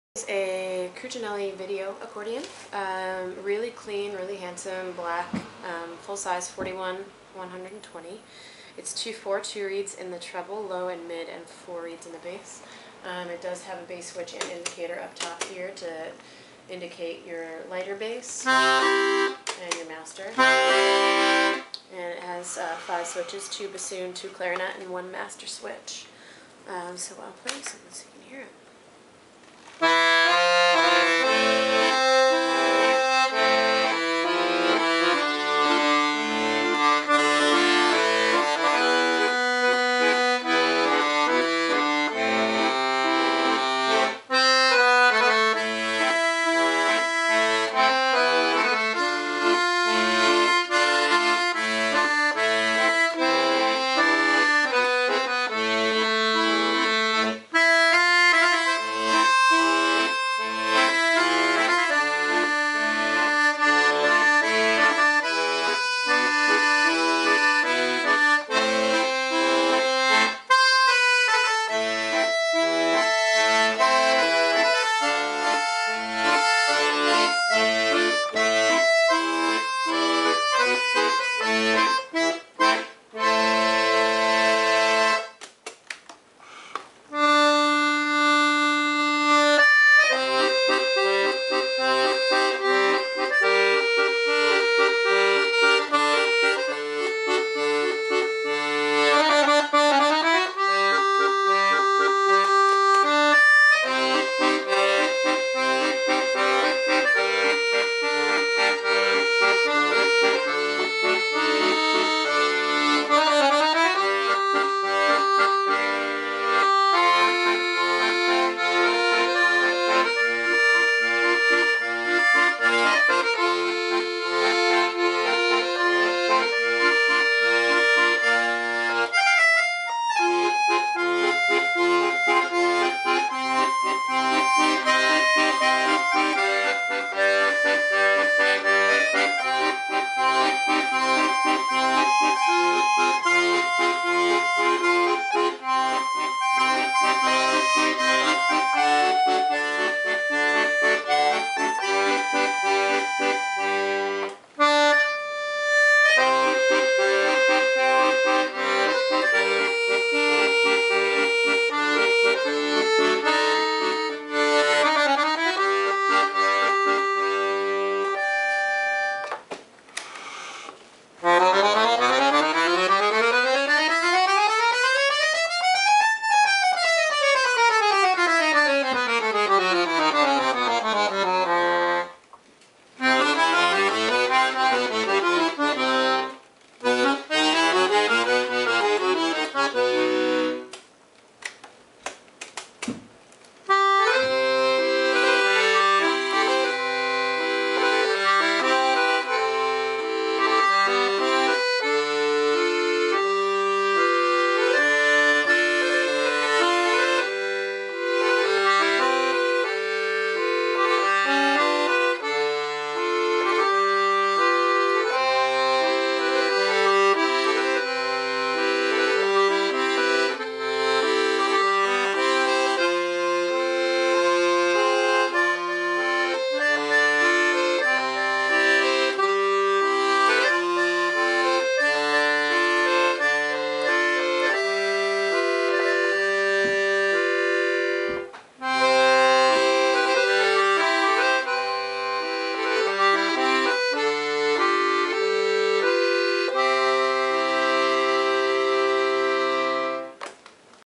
505 - Black Video LM 120 $595
DETAILS:
Black Video Crucianelli Pancordion
Piano Accordion 19.5" 41/120 21 lbs
Reeds 2/4 LM, Registers 5/2
Pristine condition